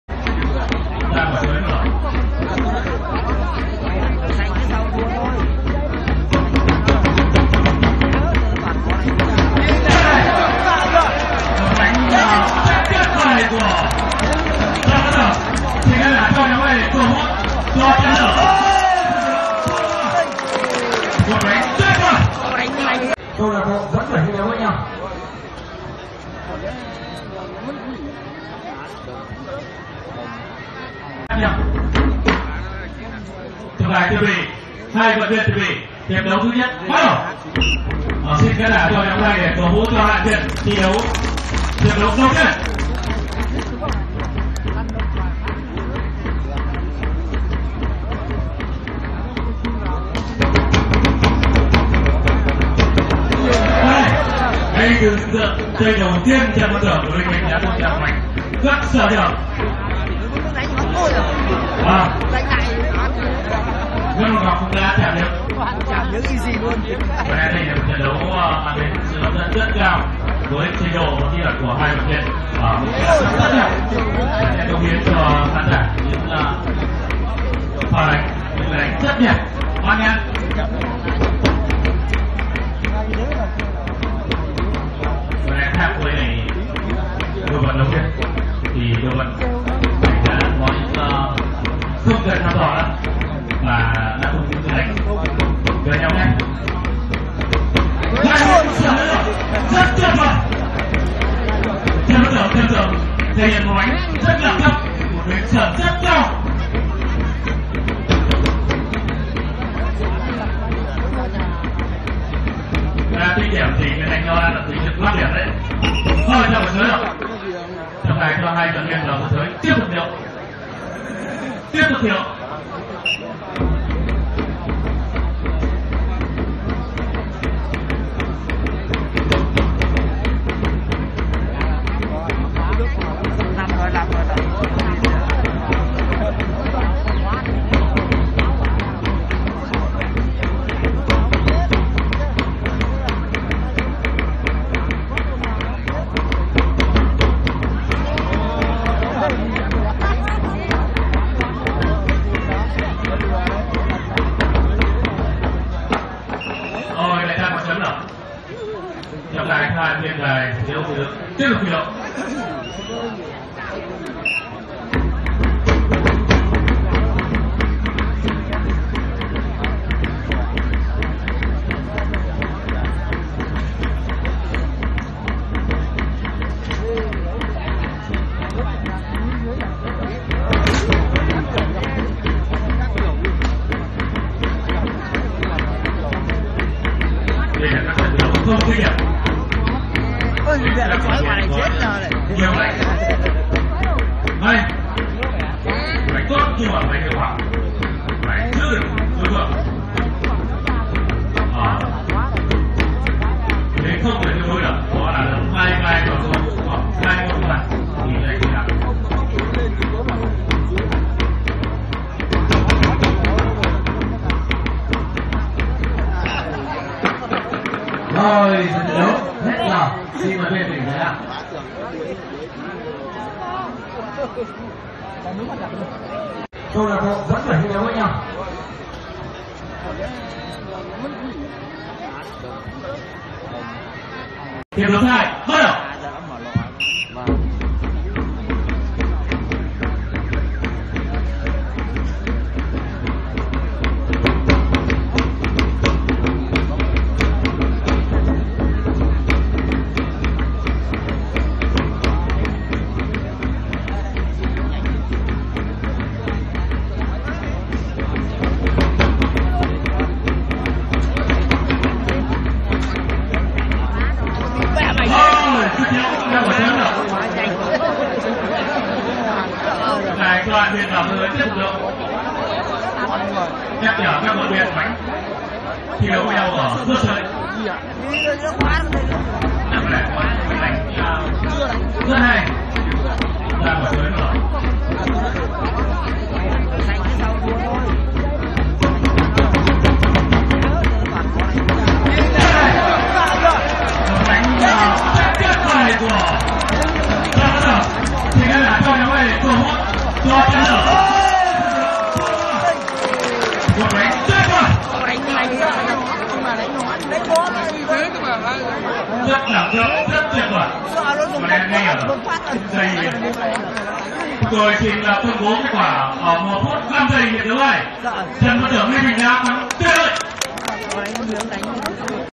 The male wrestler in the red pants made a great strike and won | lucha libre
The male wrestler in the red pants made a great strike and won | lucha libre | Freestyle Wrestling | Wrestling